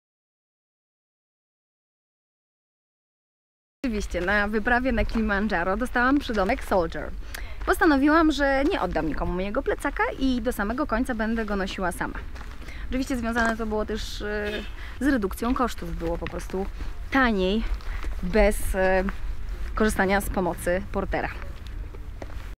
Miłka Raulin zdradza sekret swojego przezwiska | Bank Pekao S.A.
Jak brać życie za rogi, to tylko z Bankiem Pekao S.A. - sprawdź na
Nic tak nie motywuje do działania jak zbliżający się cel.
Wyprawa Miłki Raulin na Kilimandżaro wymagała wielkiego wysiłku.
Skąd dokładnie wziął się więc przydomek Miłki: „soldier”?